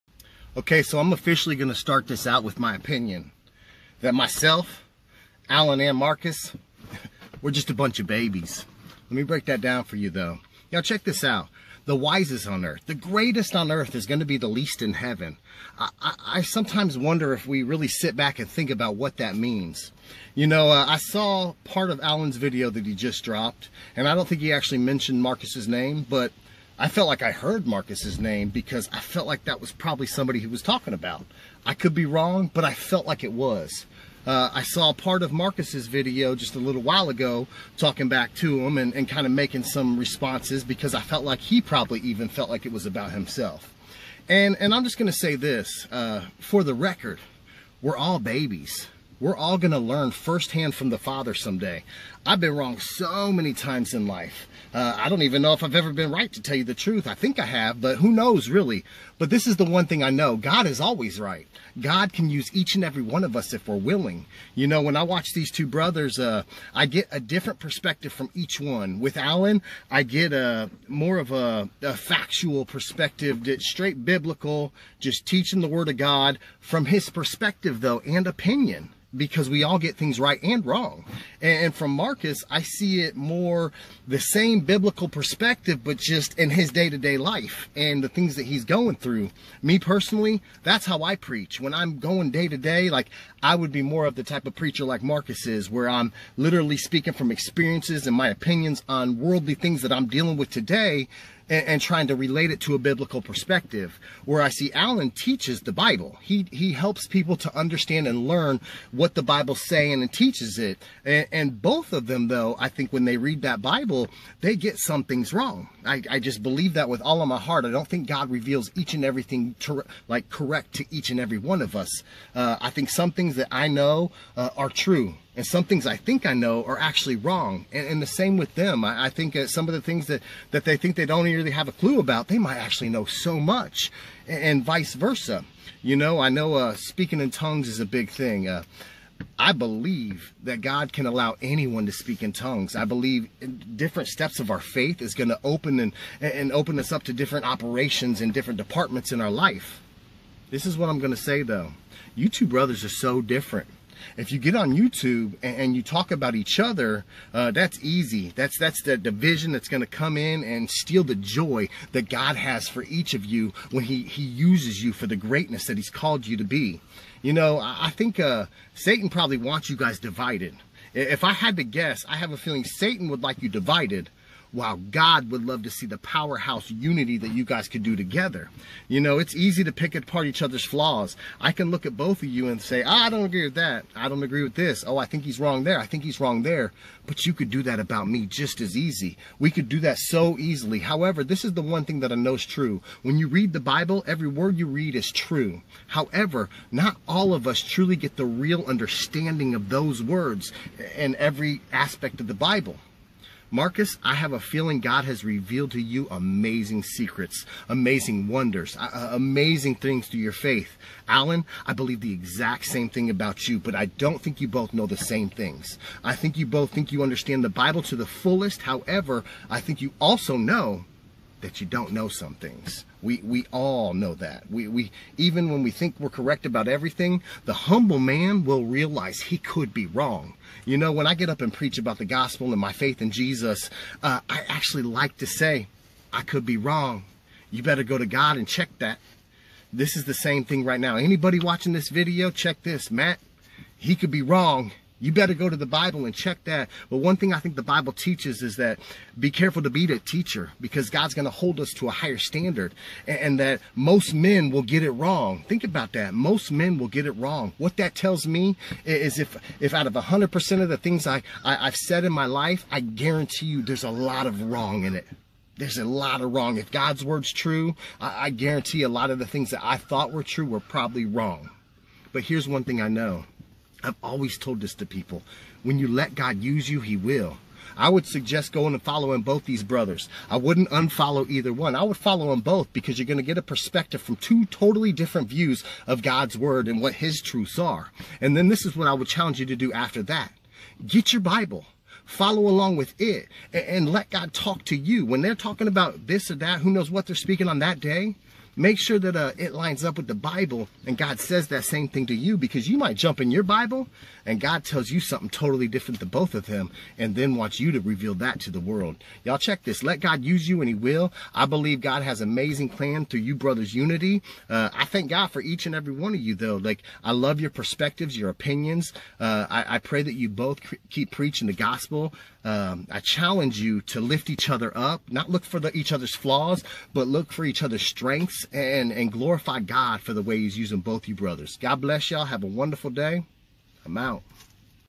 Allen Parr, Marcus Rogers, and Matt Bold are ALL BABIES! (HAD TO BE SAID)
CHECK IT: Drop an "AMEN" below this video if you know GOD CAN USE ANYONE! Real Talk. We all get things wrong and can easily find each others flaws however we could even be wrong then! Both preach from a different perspective that could be beneficial to helps us as well better realize we have to also seek Gods word ourselves to make sure HE aligns in our spirit what is being preached.. AMEN!